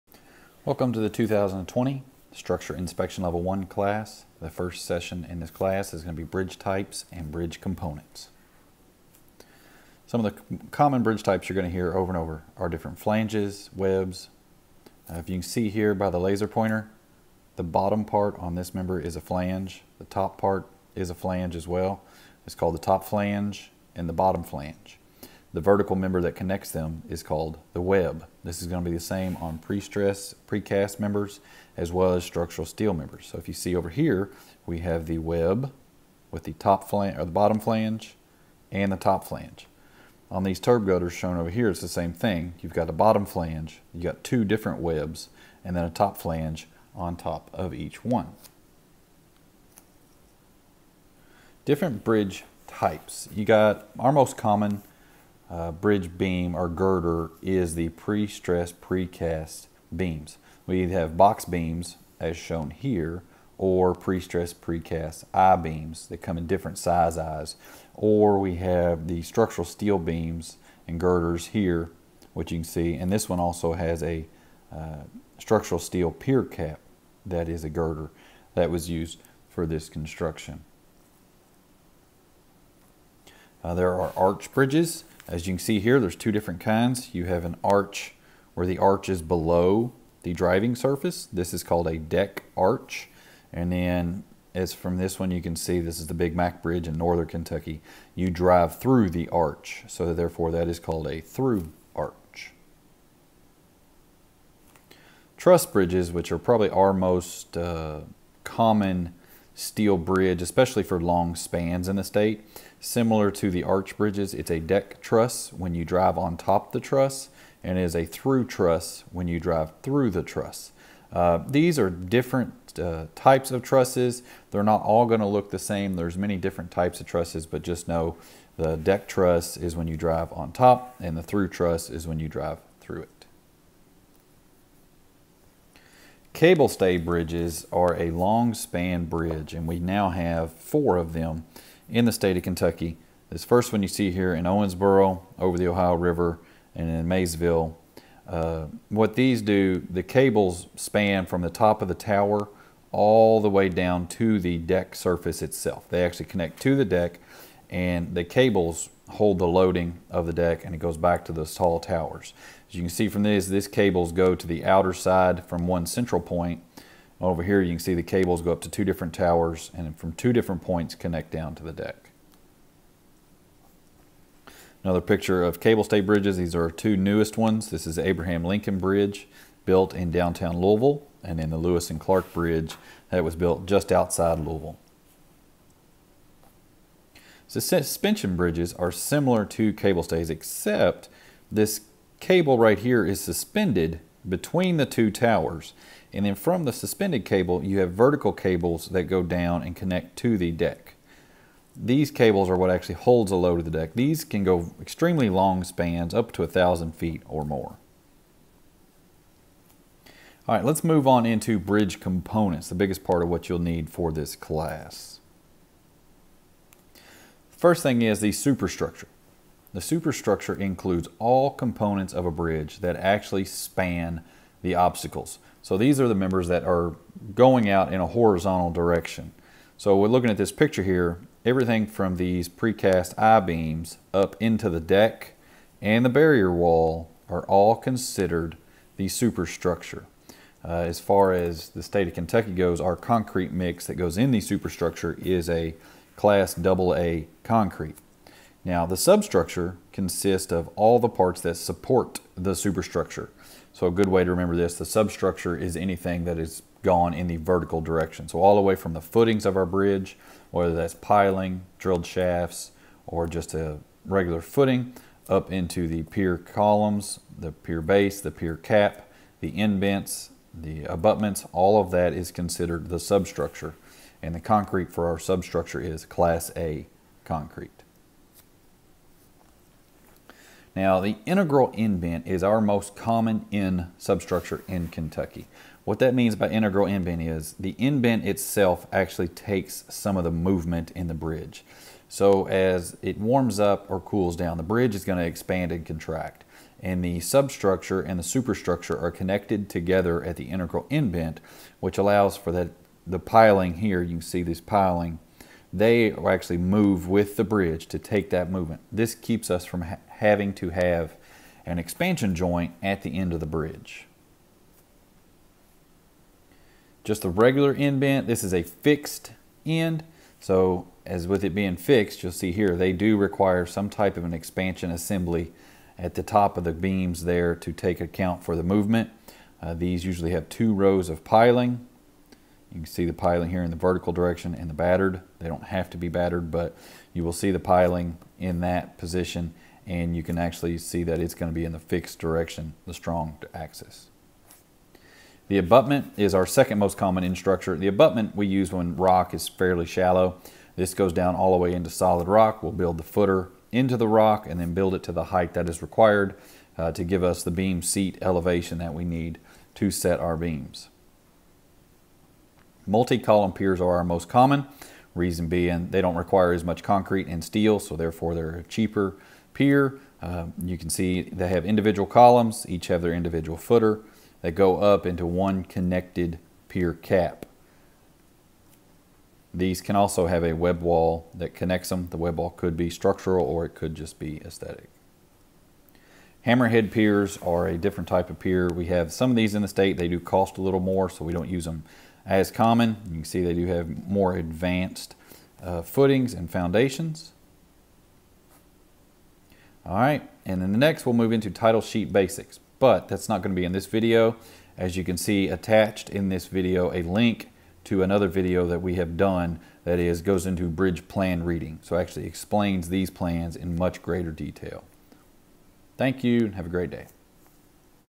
KYTC Bridge Types and Bridge Components
Session 1 for the KYTC Structure Inspection Level 1 class.